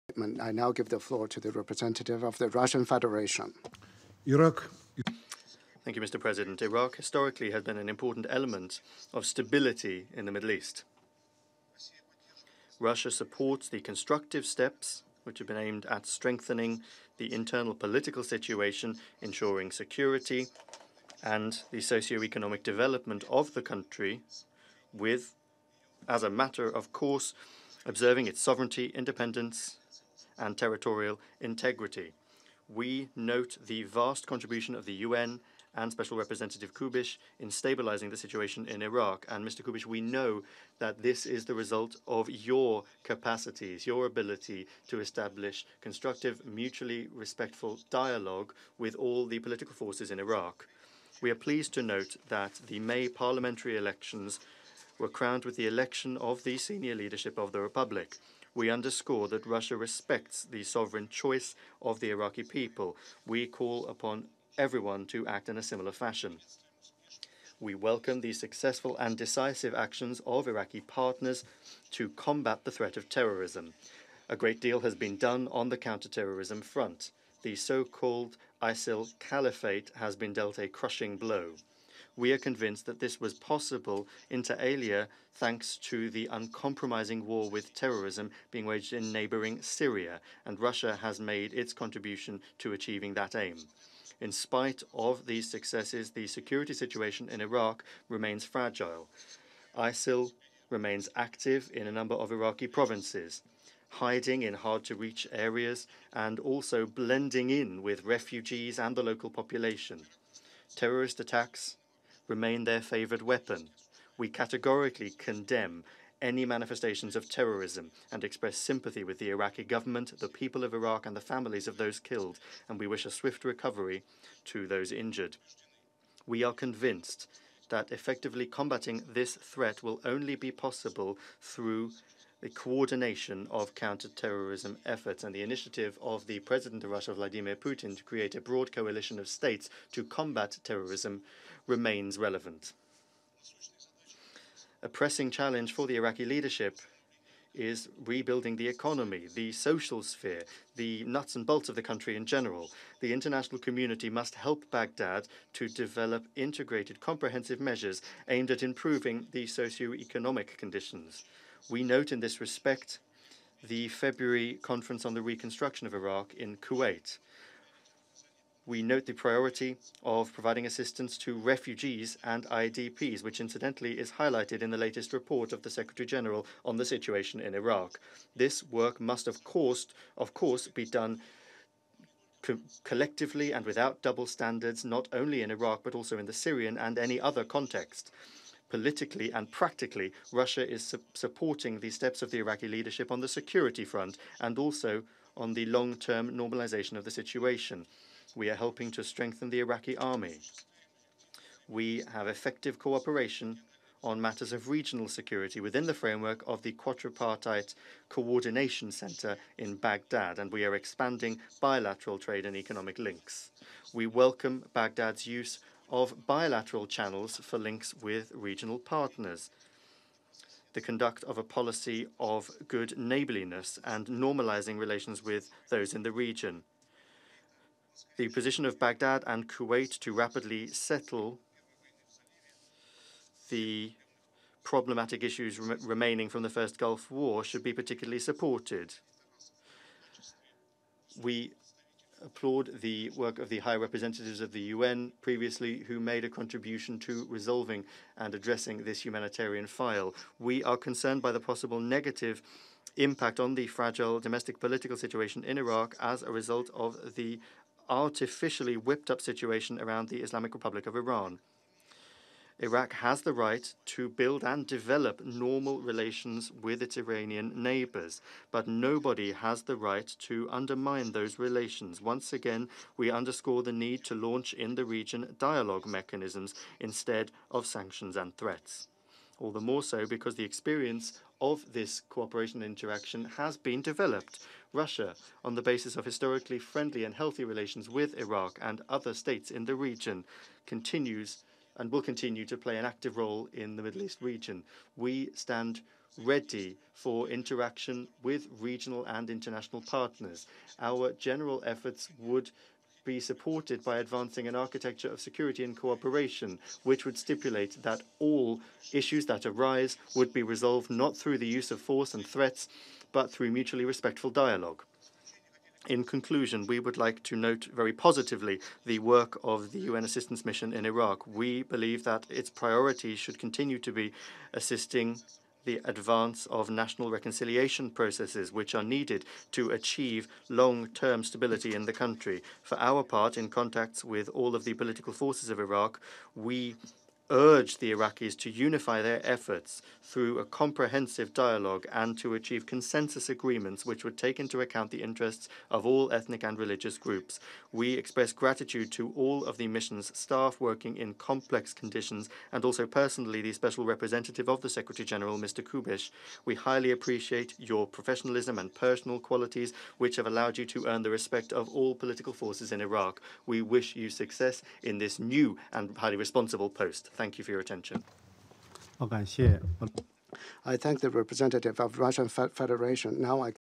Statement by DPR Vladimir Safronkov at the UNSC on the situation in Iraq
Statement by DPR Vladimir Safronkov at the UNSC on the situation in Iraq, November 13, 2018